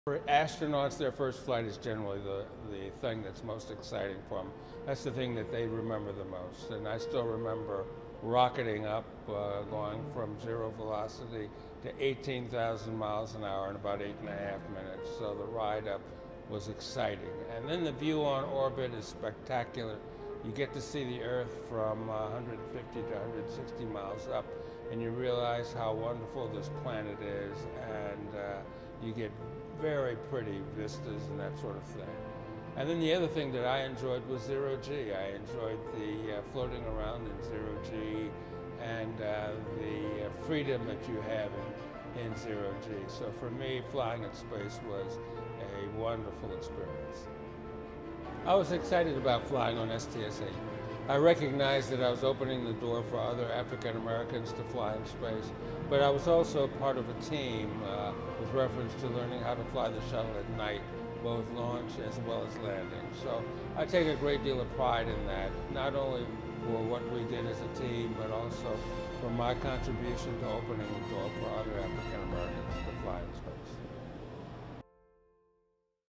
NASA | Guy Bluford Reflects on the 35th Anniversary of His First Space Flight
On August 30, 1983, mission specialist Guion "Guy" Bluford became the first African-American astronaut to fly in space. The dramatic night launch of space shuttle Challenger on the STS-8 mission 35 years ago today, marked an important milestone in American history. Bluford, who flew on three more shuttle missions during his NASA career, reflects on the significance of his first space flight.